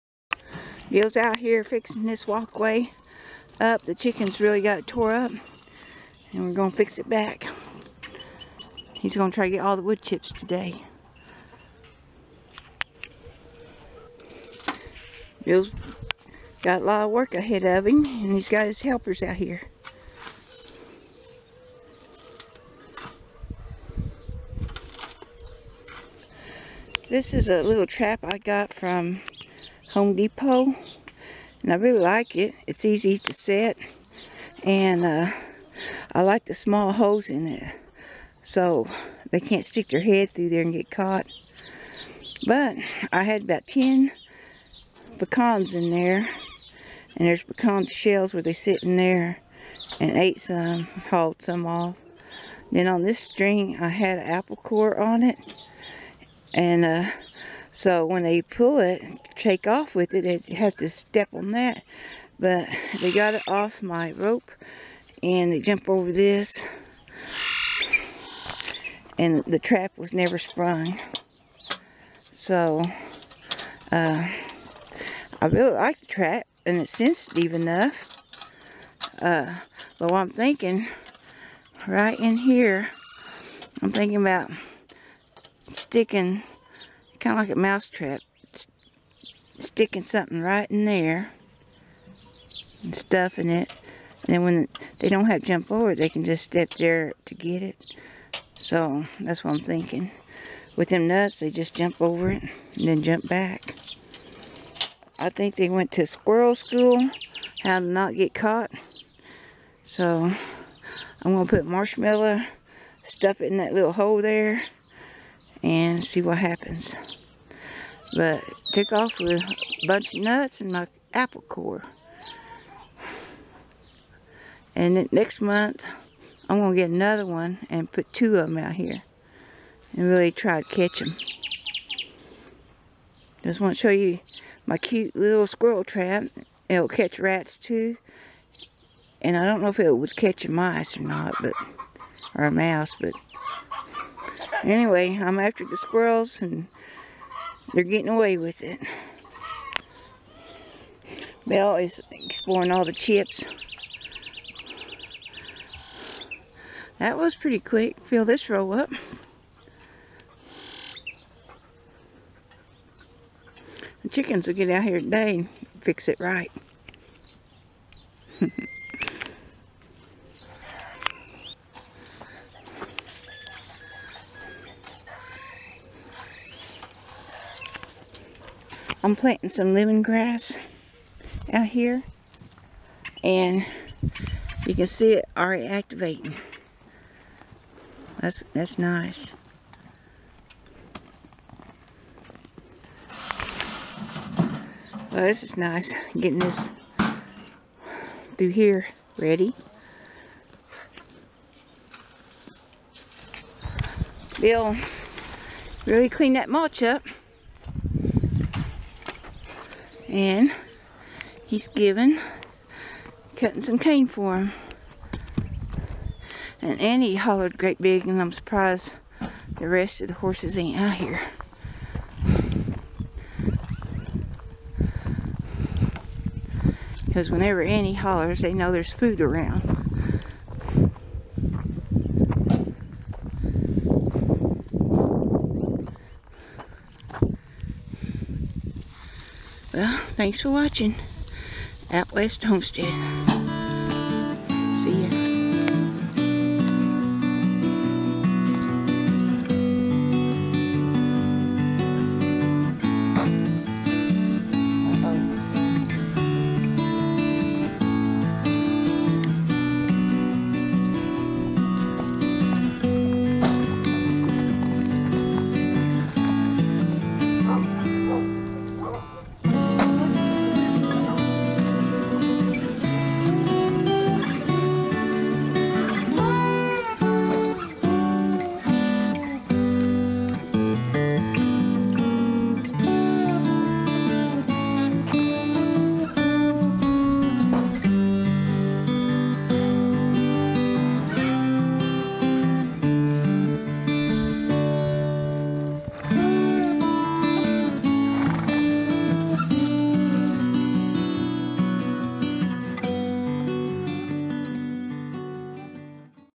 Moving mulch
It was a lot of work but Bill got the mulch moved. He's trying to get stuff done so he can get back to work on the front porch.